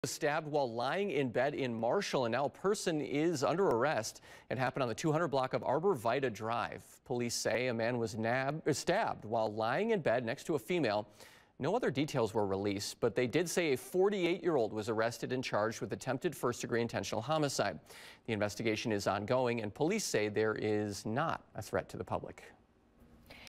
Marshall Police: Woman charged with attempted first-degree intentional homicide following domesti...
A 48-year-old woman was arrested and charged with attempted first-degree intentional homicide after she allegedly stabbed a man that was lying in bed.